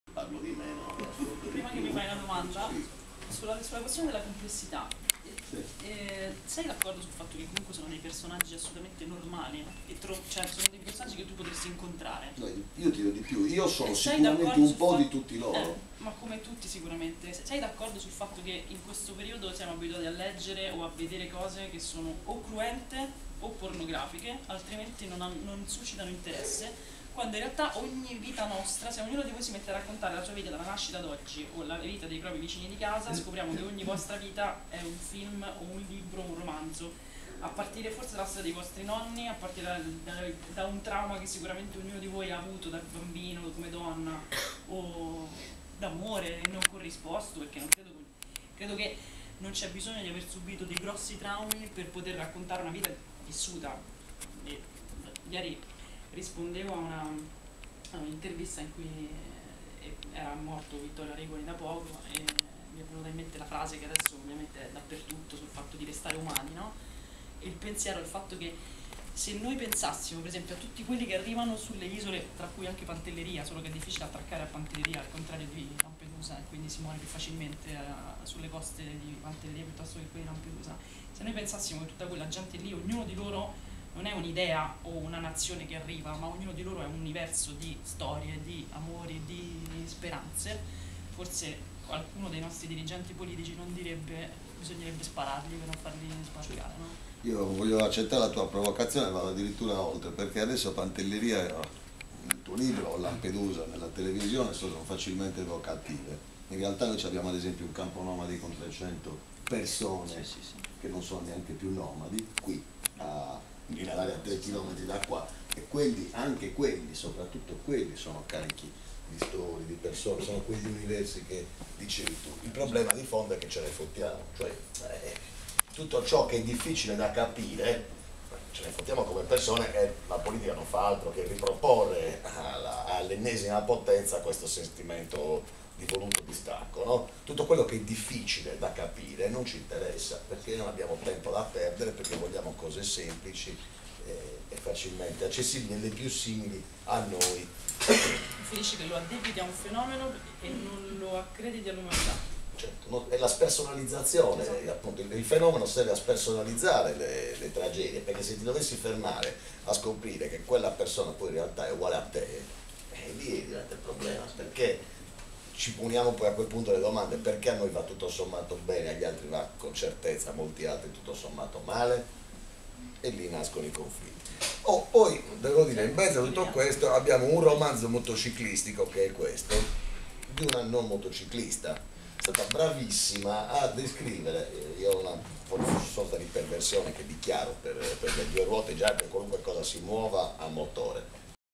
Cristiana Alicata presenta a Cagliari il suo nuovo romanzo (2)
Cristiana Alicata ha presentato a Cagliari, presso la Libreria Piazza Repubblica, il suo secondo romanzo, intitolato: "Verrai a trovarmi d'inverno".